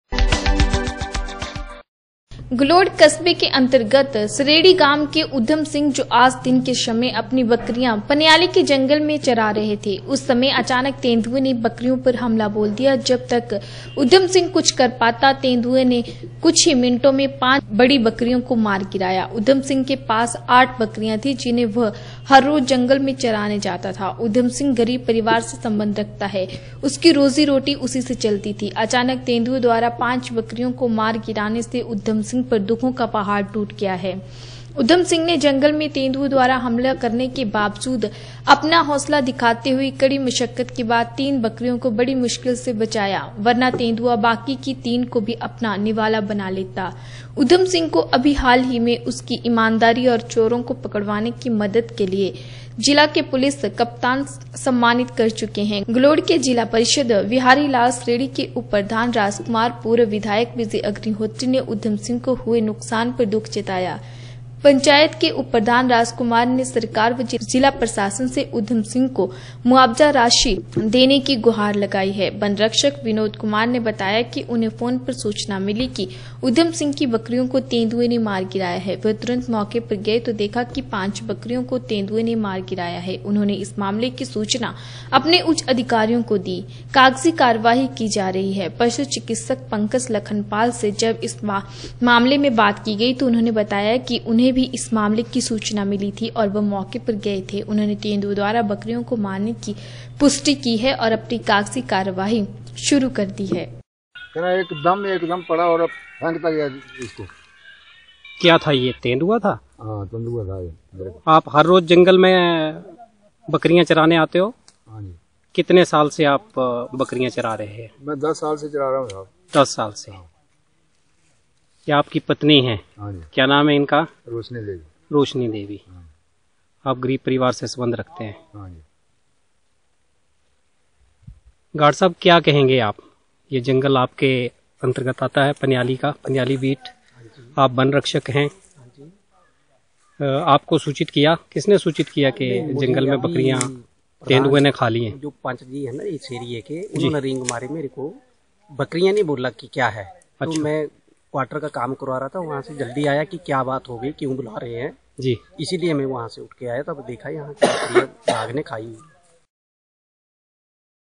गलोड कस्वे के पनियाली के जंगल मेंतेंदुए द्वारा 5 बकरियों को मार गिरा
गलोड कस्वे  के अंतर्गत सरेडी गांव के उधमसिंह  दिन के समय अपनी बकरियां पनियाली के जंगल में चरा रहे थे  उस समय अचानक तेंदुए ने बकरियों पर हमला बोल दिया जब तक उधमसिंह कुछ समझ पाता तेंदुए ने कुछ ही मिनटों में पांच बड़ी बकरियों को मार गिराया  उधम सिंह के पास 8 बकरियां थी जिन्हें बह   हर रोज जंगल में चराने जाता था उधमसिंह गरीब परिवार से संबंध रखता है उसकी रोजी.रोटी उसी से चलती थी अचानक तेंदुए द्वारा 5 बकरियों को मार गिराने से उधम सिंह पर दुखों का पहाड़ टूट पड़ा है